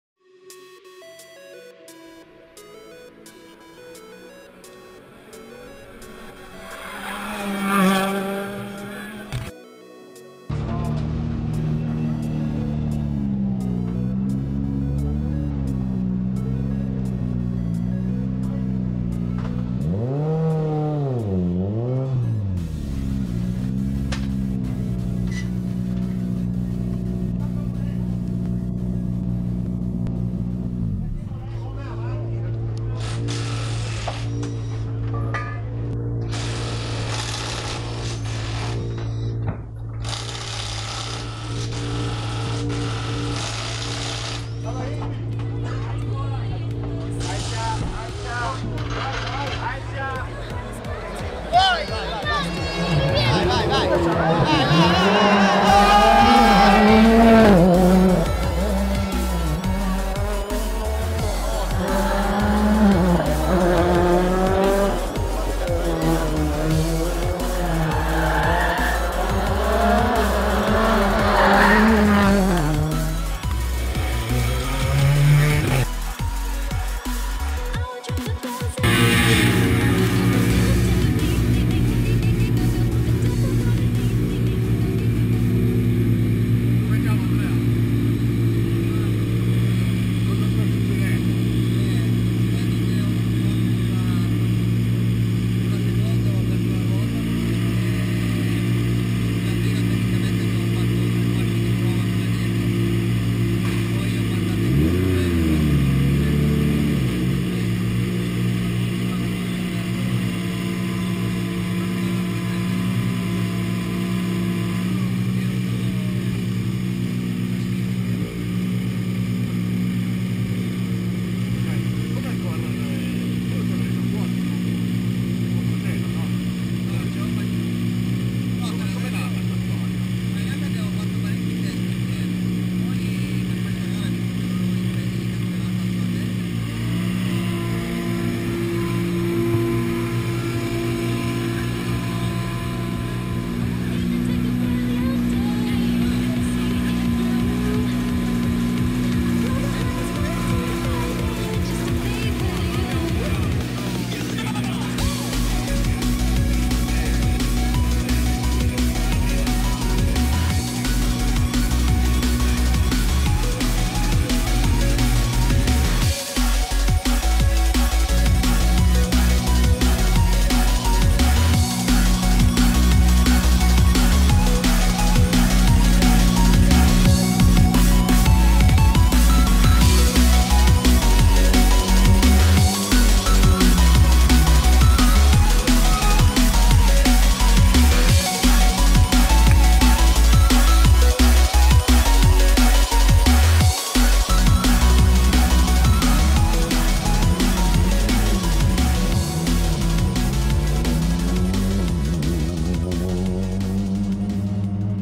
63° Rally Sanremo ANDREA NUCITA GIUSEPPE NUCITA
63° Rally Sanremo seconda tappa del campionato Italiano Rally CIR l' equipaggio NUCITA-NUCITA si classifica 4° assoluto su Ford Fiesta RRC del Team Delta Rally
Passaggi su PS4 PS10 PS11 parco assistenza e riordino con intervista.
0:04 PS4 bRIGNONE
0:11 ASSISTENZA NOTTURNA
0:51 PS5 SAN ROMOLO
1:01 PS6 BAJARDO
1:25 RIORDINO CON INTERVISTA
2:48 PURE SOUND
3:15 VERSO IL PALCO 4° ASSOLUTO